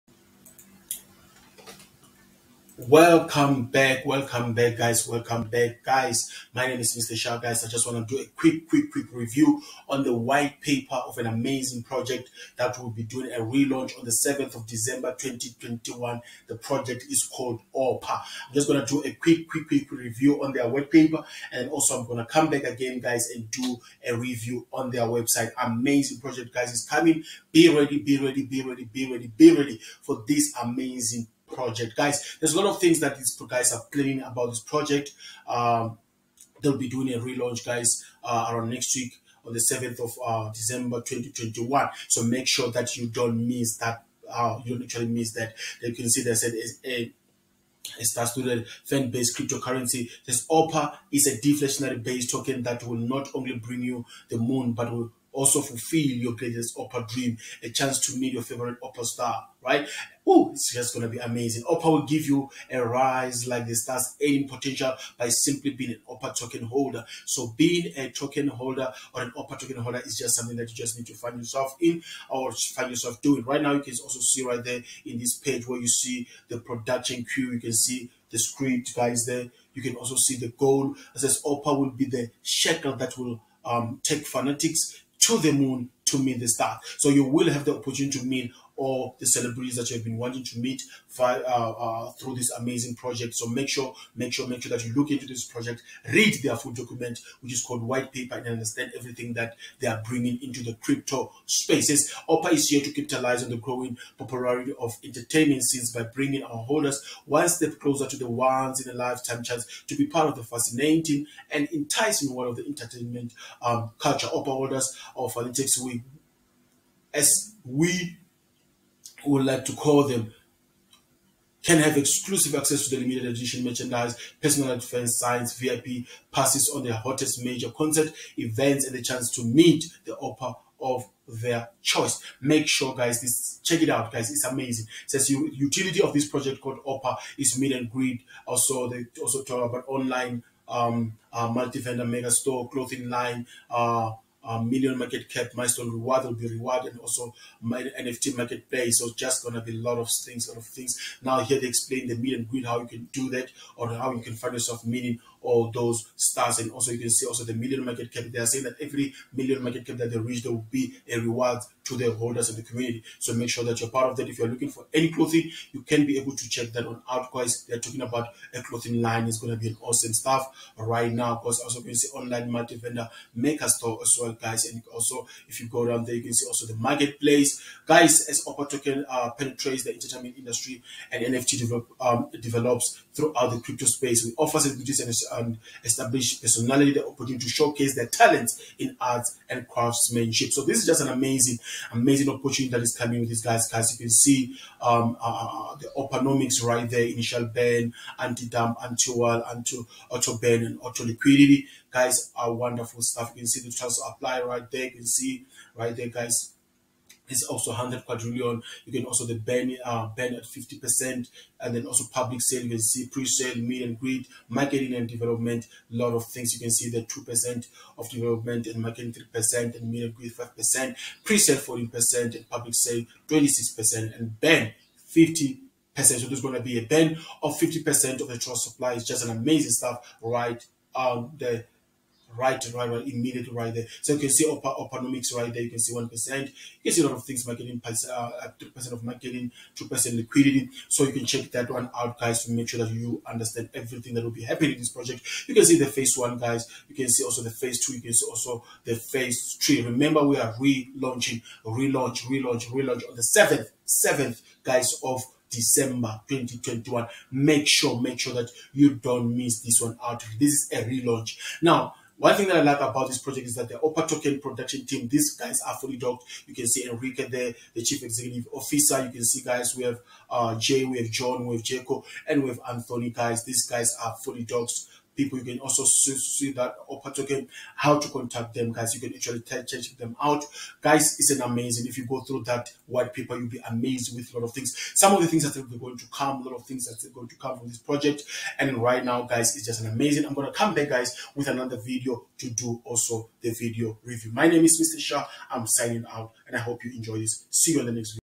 OPPA Token Whitepaper Review.....
OPPA Token is a deflationary star-studded fan-based cryptocurrency that offers a “rise-like-the-stars” earning potential. OPPA Token is committed to bring our “Fanatics” a step closer to meet their favorite celebrities.

MrShaa Links